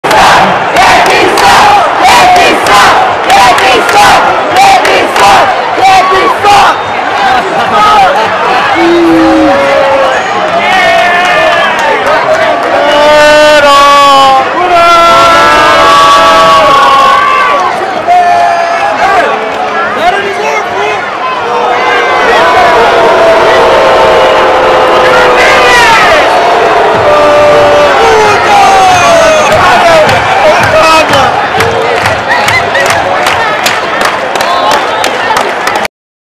yankees sock!@dodgerstadium.mp4
I am a yankeesfan.

I got good booing from dodgersfans with him.